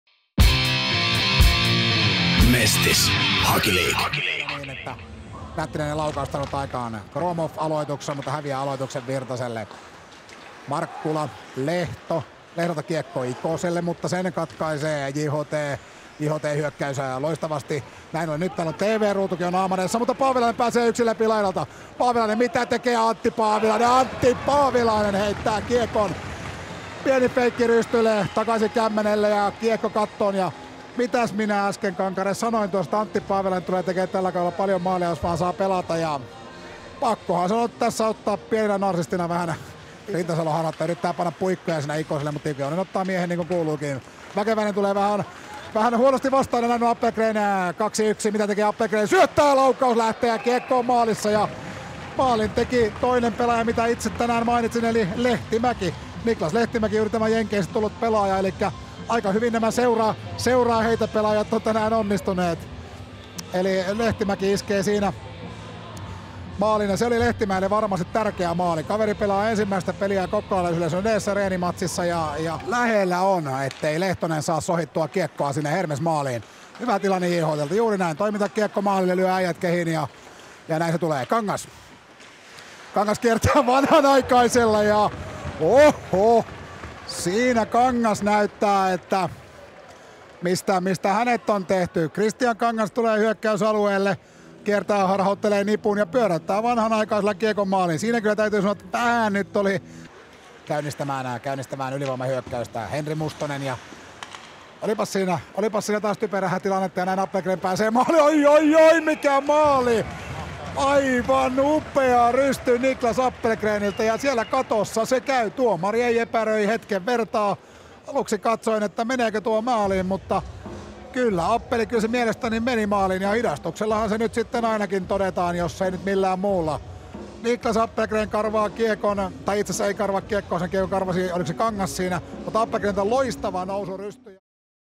Hermes - JHT pe 19.8.2016 - Maalikooste
Harjoitusottelun maalikooste.

1-0 25 Antti Paavilainen
2-0 20 Niklas Lehtimäki
3-0 26 Kristian Kangas
4-0 22 Niklas Appelgren